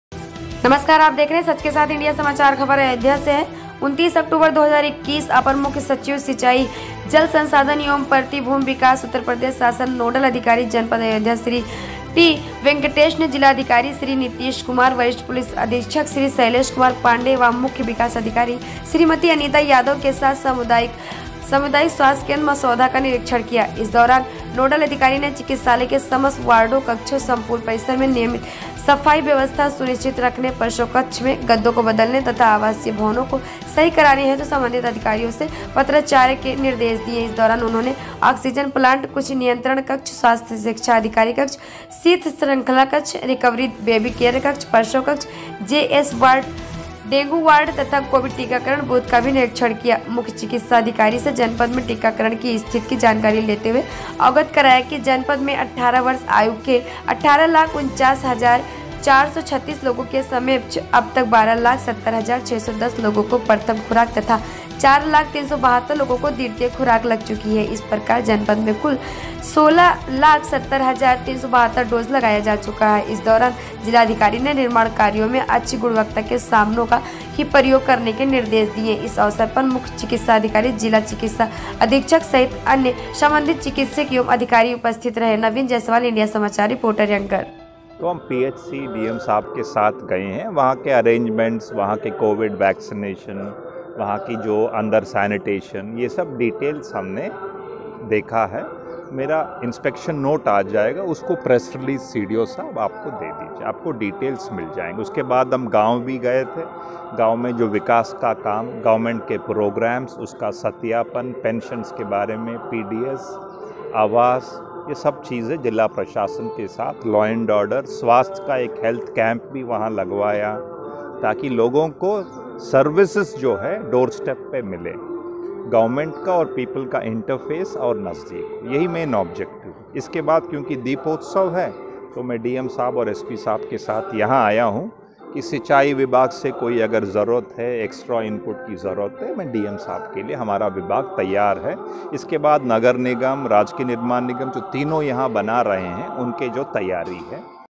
अपर मुख्य सचिव सिंचाई, जल संसाधन एवं परती भूमि विकास उ0प्र0 शासन/नोडल अधिकारी जनपद अयोध्या श्री टी0
*अयोध्या 29 अक्टूबर 2021 (सूवि)ः*-अपर मुख्य सचिव सिंचाई, जल संसाधन एवं परती भूमि विकास उ0प्र0 शासन/नोडल अधिकारी जनपद अयोध्या श्री टी0 वेंकटेश ने जिलाधिकारी श्री नितीश कुमार, वरिष्ठ पुलिस अधीक्षक श्री शैलेश कुमार पांडेय व मुख्य विकास अधिकारी श्रीमती अनीता यादव के साथ सामुदायिक स्वास्थ्य केन्द्र मसौधा का निरीक्षण किया। इस दौरान नोडल अधिकारी ने चिकित्सालय के समस्त वार्डो/कक्षों, सम्पूर्ण परिसर में नियमित सफाई व्यवस्था सुनिश्चित रखने, प्रसव कक्ष में गद्दो को बदलने तथा आवासीय भवनों को सही कराने हेतु सम्बंधित अधिकारी से पत्राचार के निर्देश दिये। इस दौरान उन्होंने आक्सीजन प्लांट, कुष्ठ नियंत्रण कक्ष, स्वास्थ्य शिक्षा अधिकारी कक्ष, शीत श्रृंखला कक्ष, रिकवरी वेवी केयर कक्ष, प्रसव कक्ष, जे0एस0वाई0 वार्ड, डेंगू वार्ड तथा कोविड टीकाकरण बूथ का भी निरीक्षण किया। मुख्य चिकित्साधिकारी से जनपद में टीकाकरण की स्थिति की जानकारी लेने पर अवगत कराया कि जनपद में 18 वर्ष आयु के 18 लाख 49 हजार हजार 436 लोगों के सापेक्ष अब तक 12 लाख 70 हजार 610 लोगों को प्रथम खुराक तथा 04 लाख 372 लोगों को द्वितीय खुराक लग चुकी है। इस प्रकार जनपद में कुल 16 लाख 70 हजार 372 डोज लगाया जा चुका है।
 इस दौरान जिलाधिकारी ने निर्माण कार्याे में अच्छी गुणवत्ता के सामानों का ही प्रयोग करने के निर्देश दिये। इस अवसर पर मुख्य चिकित्साधिकारी, चिकित्सा अधीक्षक सहित अन्य सम्बंधित चिकित्सक एवं अधिकारी उपस्थित रहे। नवीन जयसवाल इंडिया समाचार रिपोर्टर एंकर